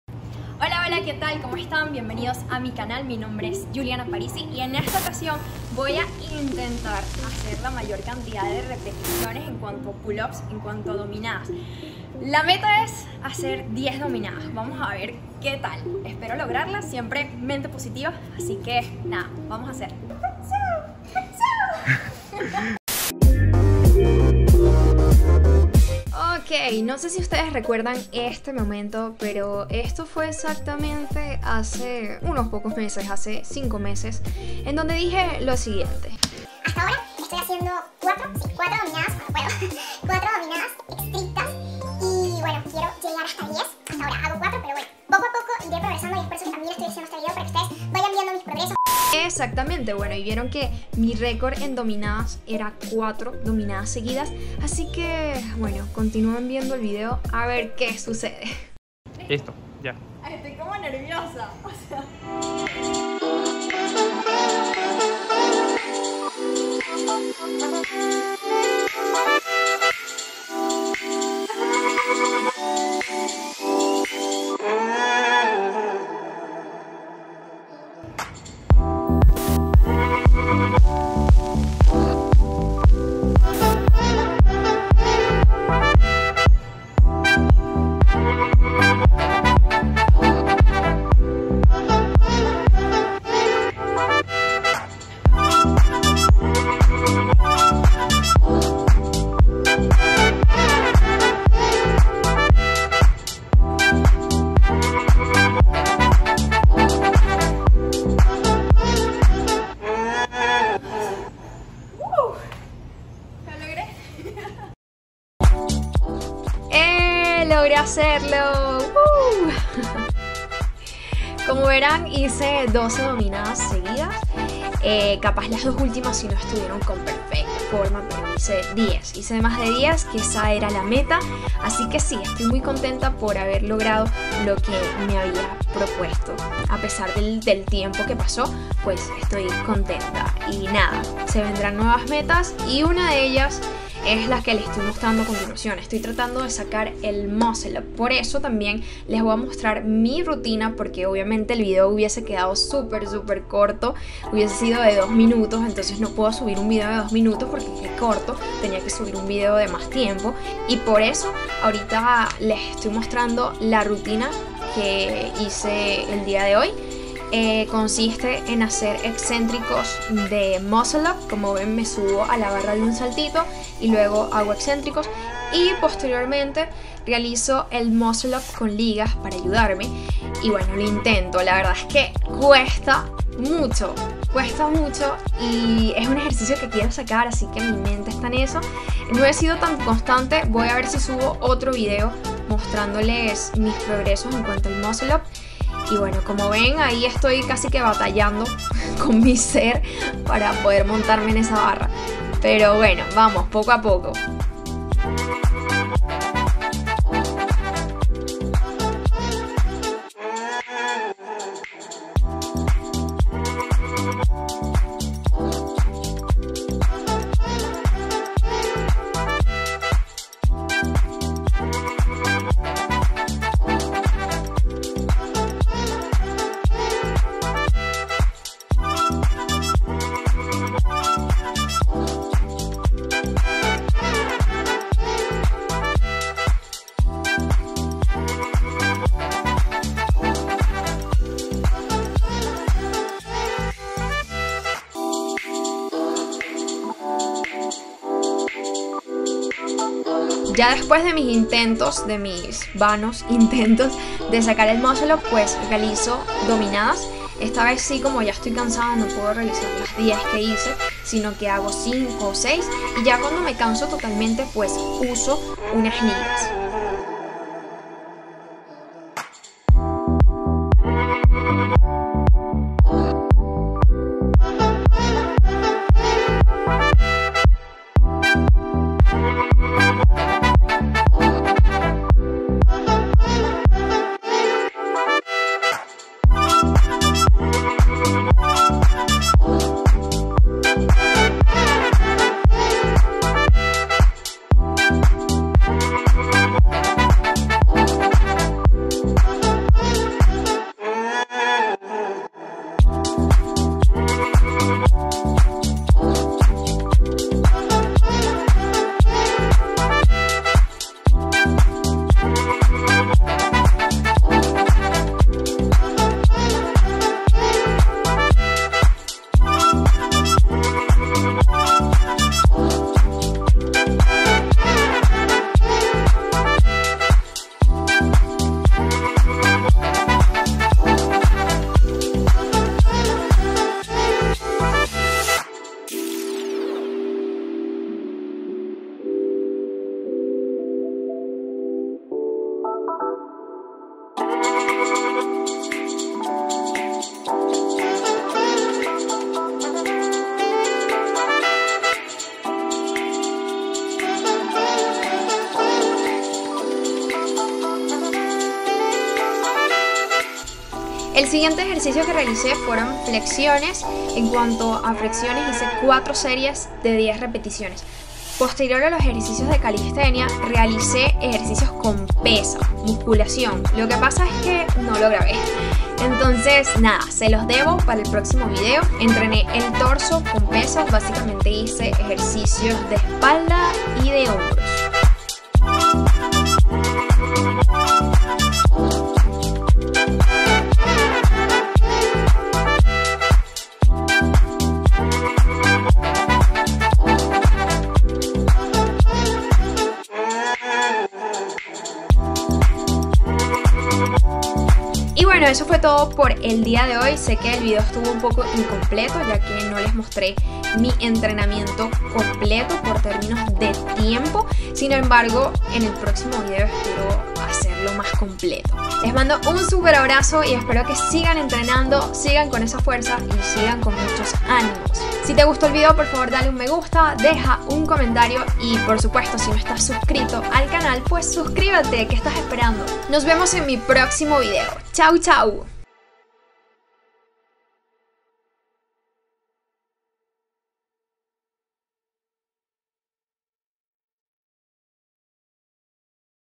¿Lograré Hacer 10 Dominadas Seguidas? ! | Juliana Parisi
¡Vamos, Vamos!